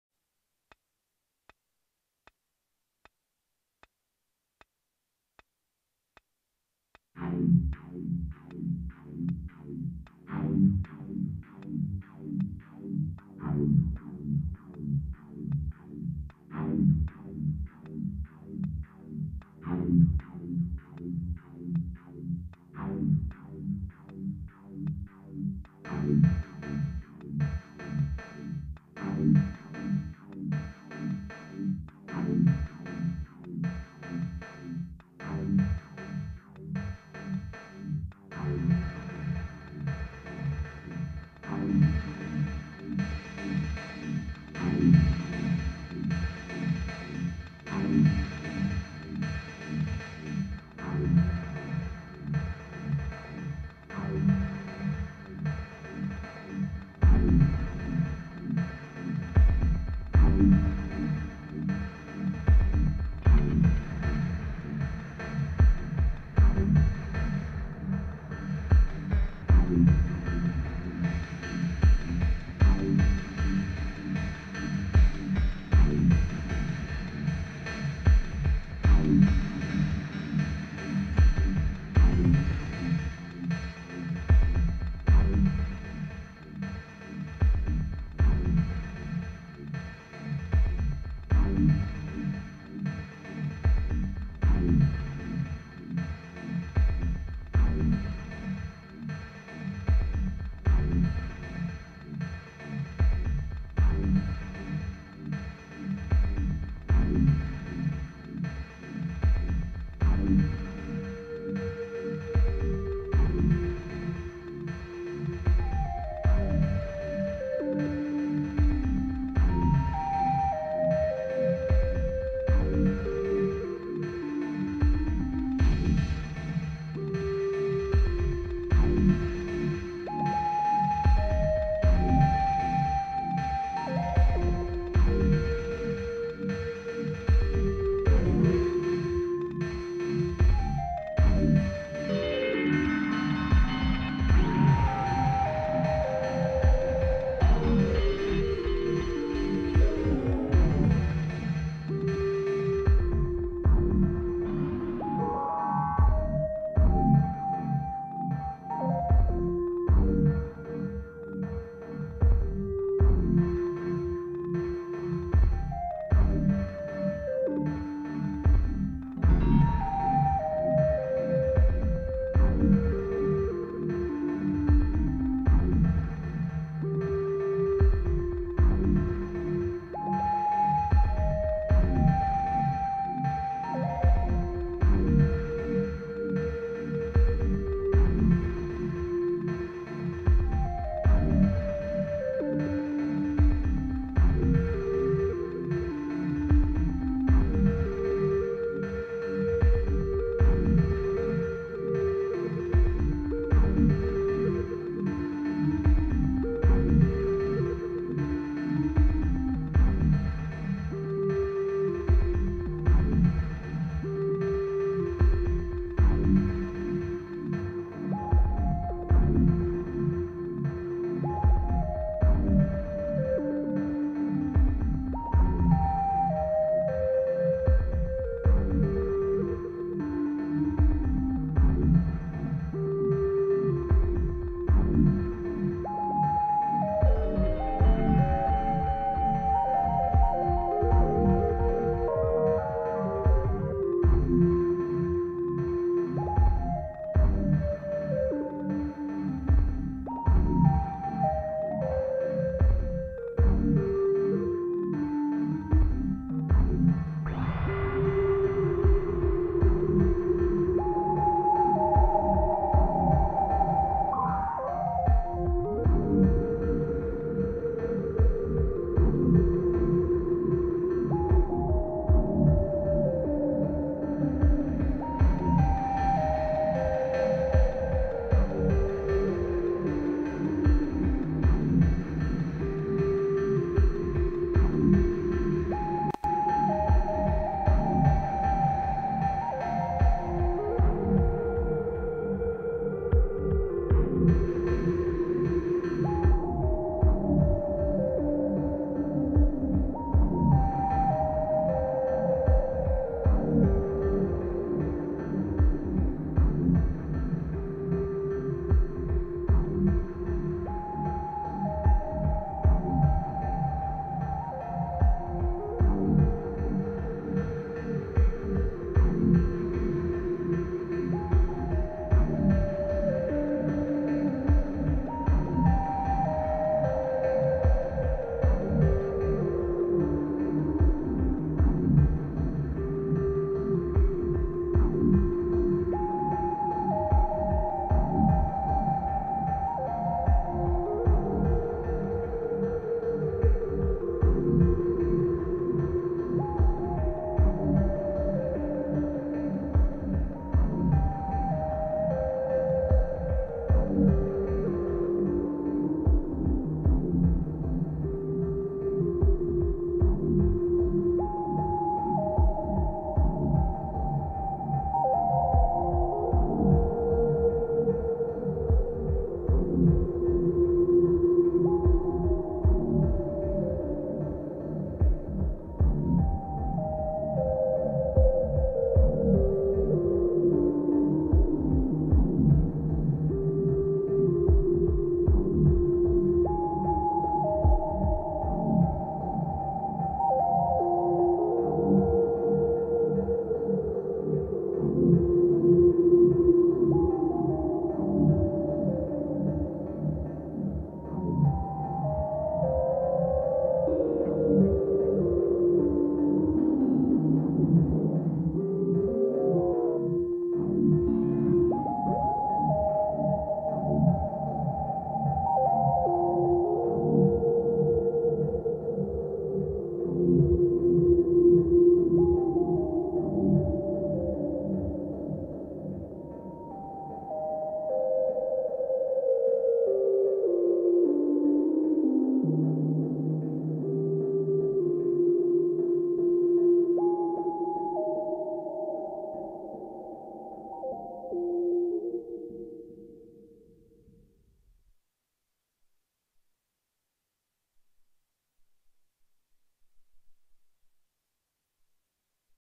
Jellyfish Dreams
this was a soundscape i made for my wife while we were on a little shroom trip. neither of us could find the right energy in the house for us to just be calm so i decided to takeout my circuit and create the energy for us. it ended up great and totally aided in an amazing trip. sadly this isn’t the original performance. i lost the video of this beauty’s creation and it really sucked. butttt i really do hope that you enjoy it! thanks for listening